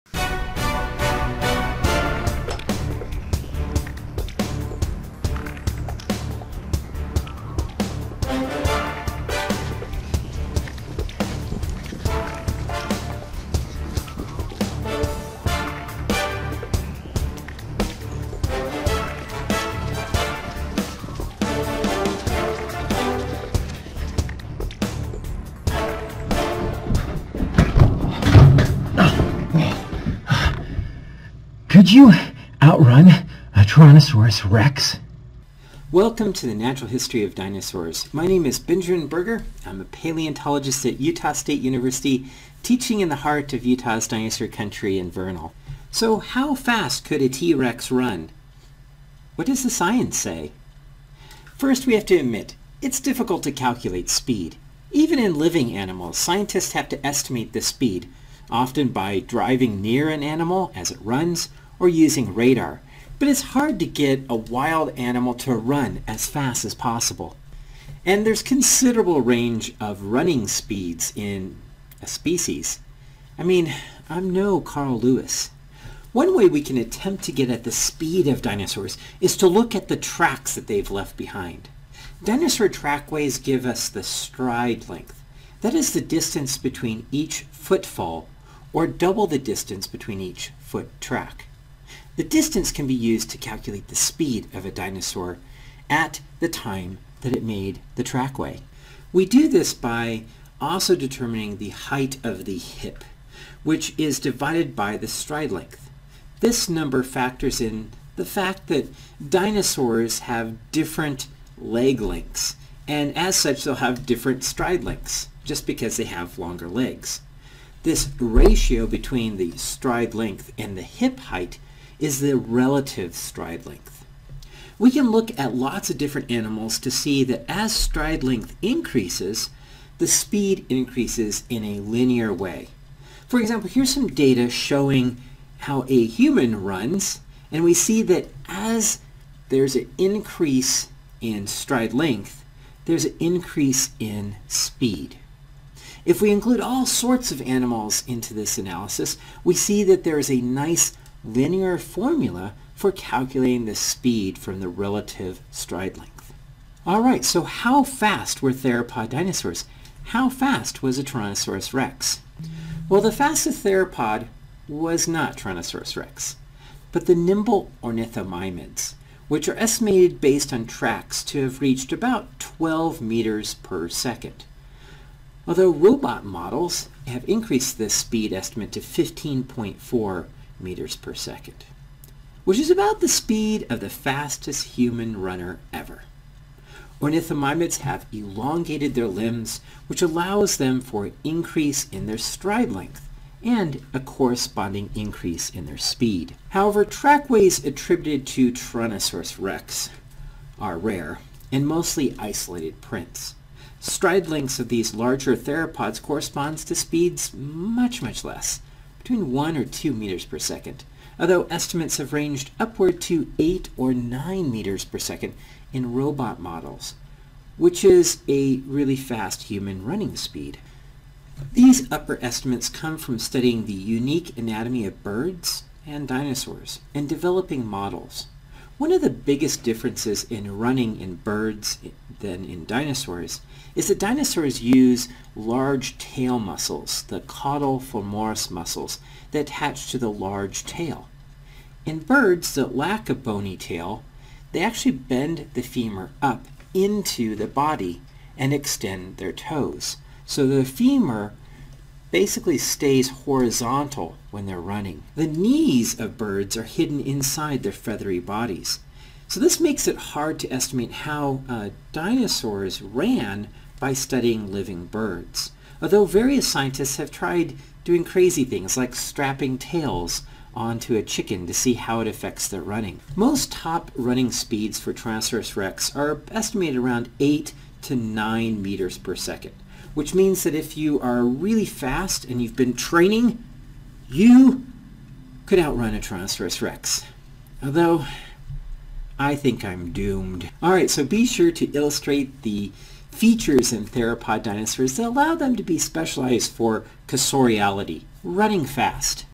Could you out run a Tyrannosaurus rex?
How do scientists estimate how fast Tyrannosaurus rex ran?